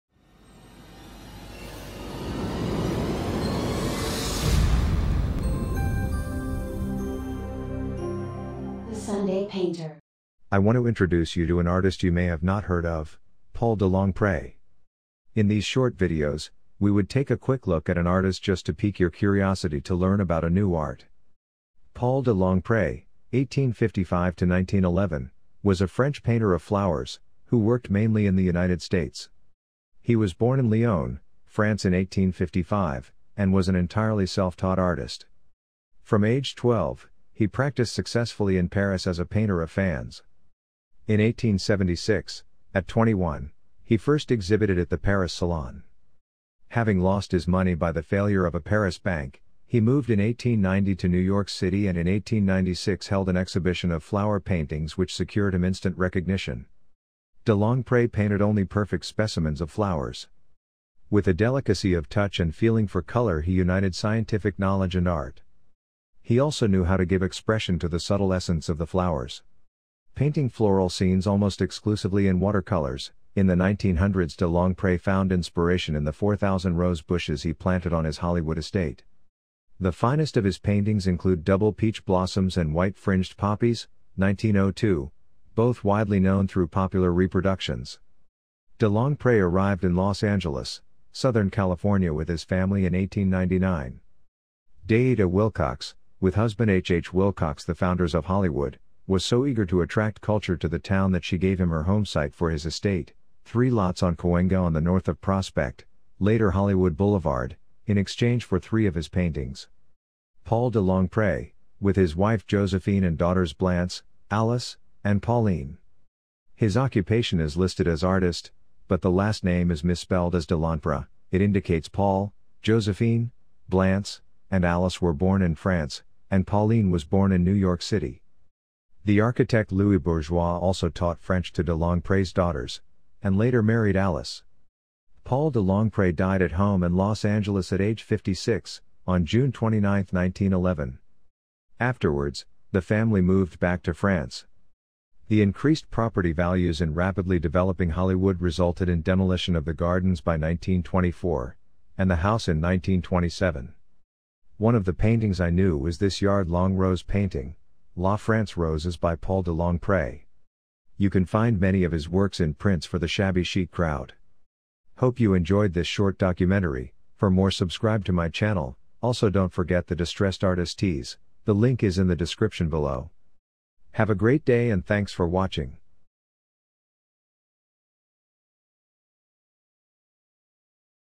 Paul de Longpré Watercolor Roses Flower Painting Style Tutorial Art History Documentary Lesson
Learn about Paul de Longpré who painted Roses and flowers in watercolor from the Artist in this short Art History Tutorial Documentary Lesson. (Shabby Chic Destressed Queen Tee